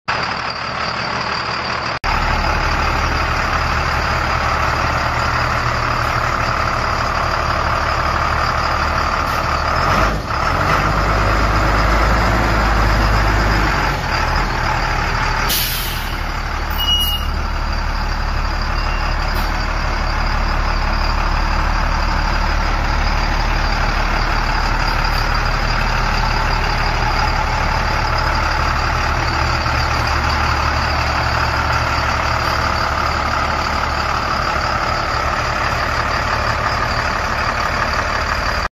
2007 International 4300 box truck for sale at auction | bidding closes December 9, 2021
2007 International 4300  box truck
 Miles: 259,839 on odometer
 VIN: 1HTMMAAL17H378976
Engine
 International DT466
 Displacement: 7.6L
 Cylinders: 6
 Fuel type: Diesel
 HP: 220
Transmission
 Automatic
Chassis
 Axles: Single
 Suspension: Spring
 Brakes: Air
 GVWR: 25,999 lbs
 Wheelbase: 255"
Interior
 AC, Heat
Operational
 Box bed
 Morgan
 24' L x 102" W x 104" H
 Floor: Wood
 End gate lift
 Maxon GPTLR33

Tires
 Size: 11R22.5
Notes
 Dash warning indicator: ABS
 Unknown operating condition of end gate lift
Illinois title
It's located in East Saint Louis, IL, and we're selling it to the highest bidder regardless of price.